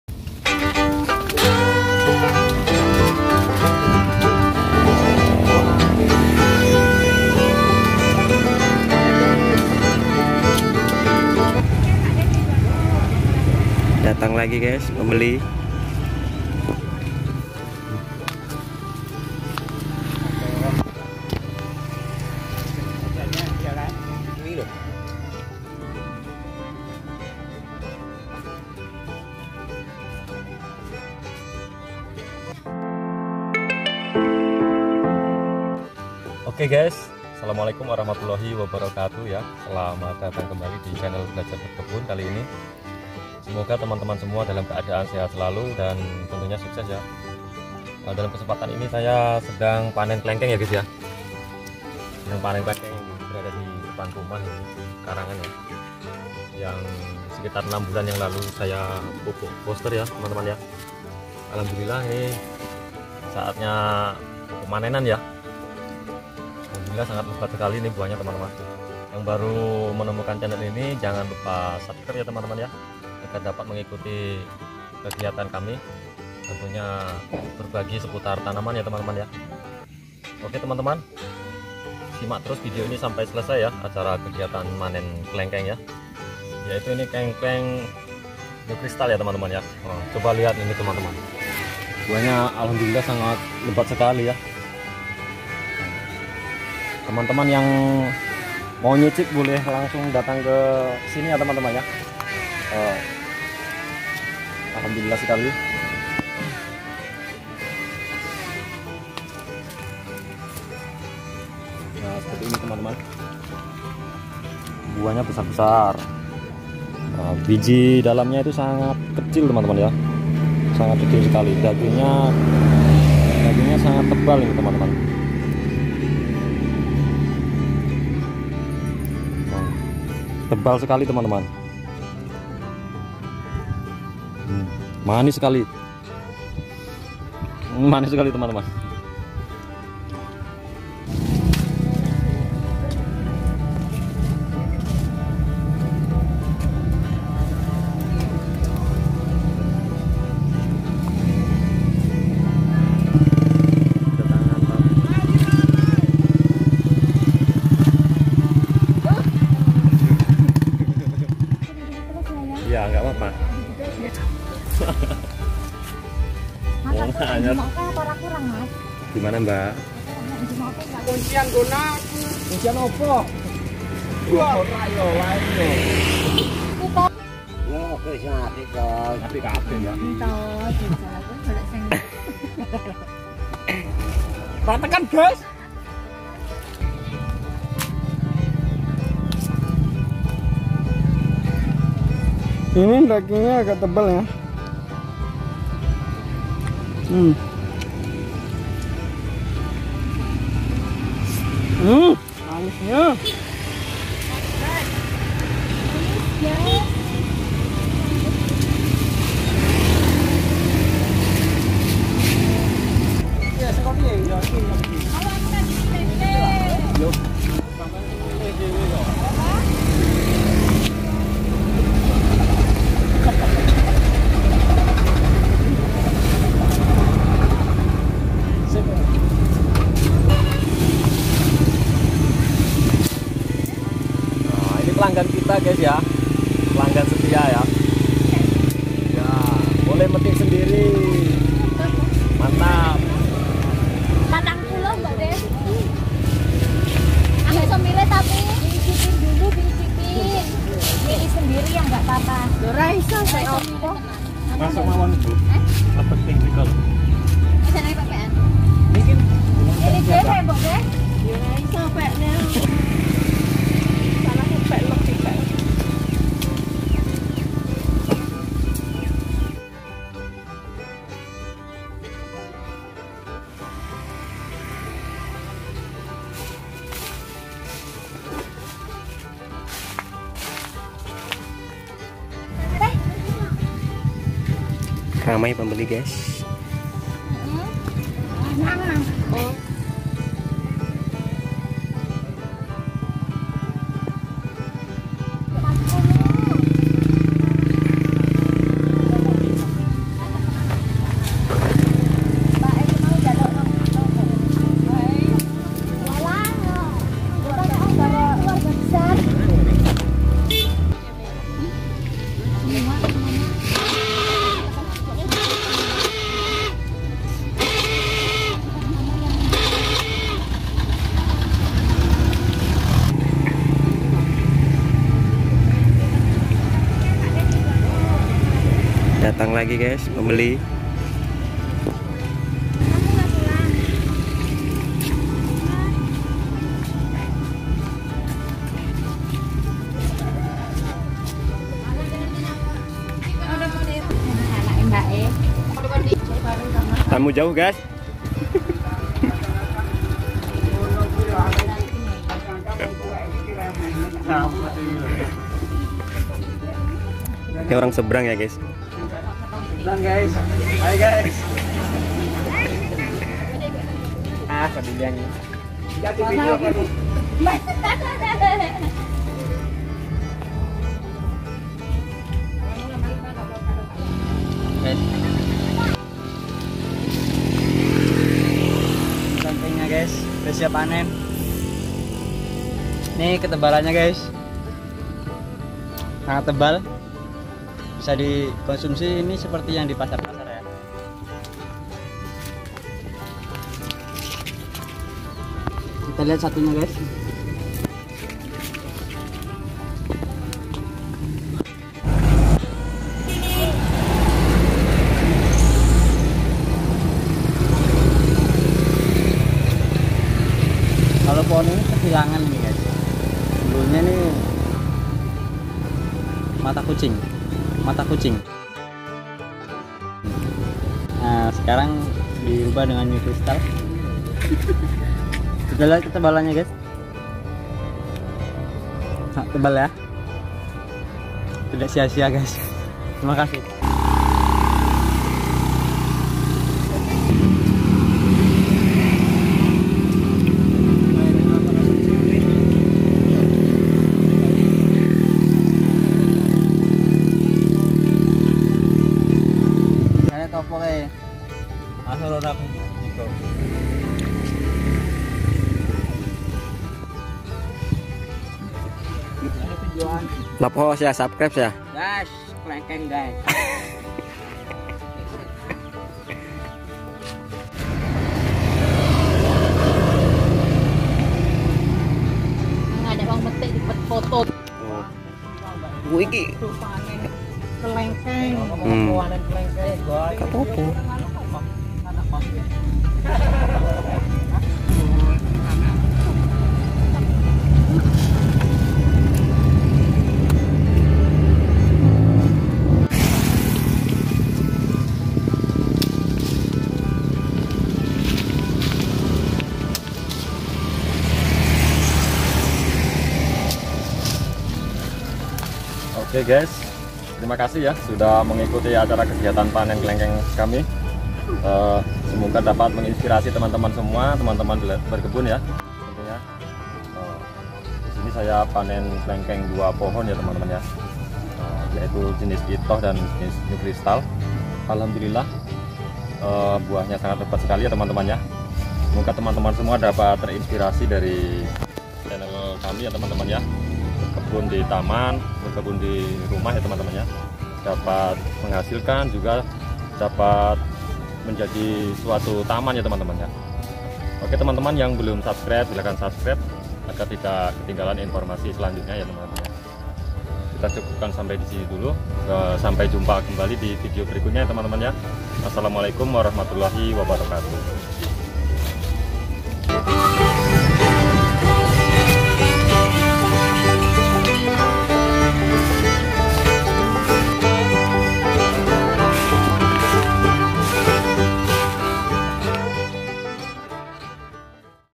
Panen Klengkeng NEW KRYSTAL SUPER,Pembeli Petik Sendiri Di Pohon.
Hallo sobat belajar berkebun,video ini kegiatan panen klengkeng new krystal super,alhamdulillah ramai pengunjung dan bebas petik sendiri.

Semoga video ini dapat menginspirasi sobat semua semakin semangat berkebun buah buahan.